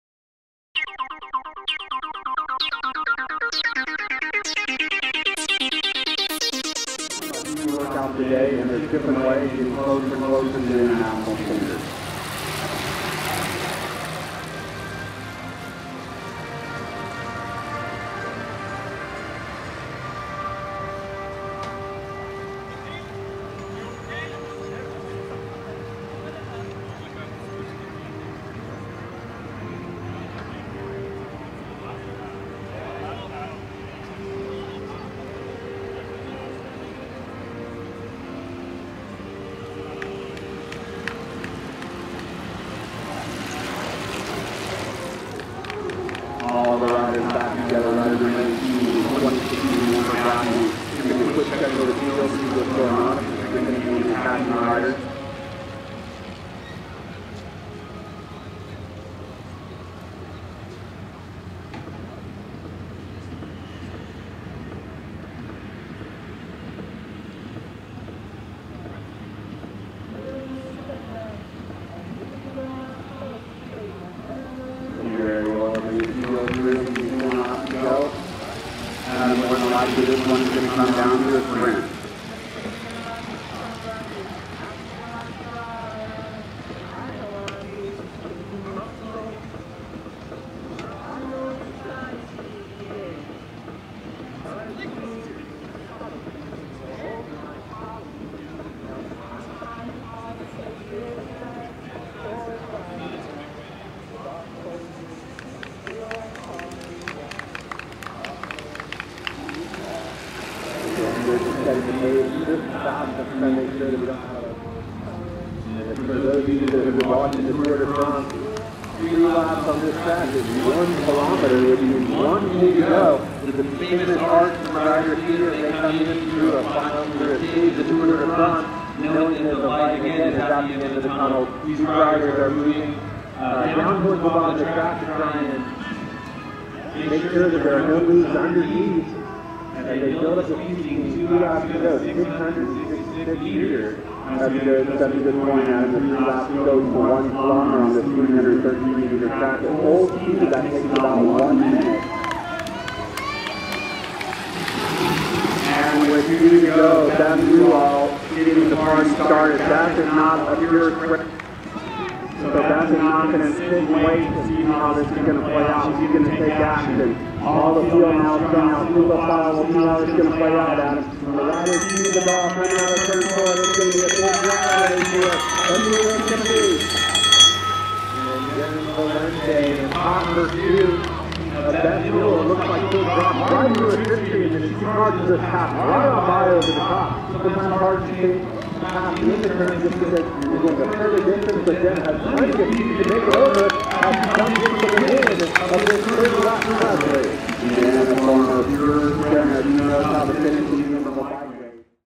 Colorado Springs Gran Prix, Women's Scratch Race 2014
Colorado Springs Gran Prix, 2014 Women's Scratch Race
June 11, 2014 at 7-11 Velodrome, Colorado Springs, Colorado
Excerpts and Finish.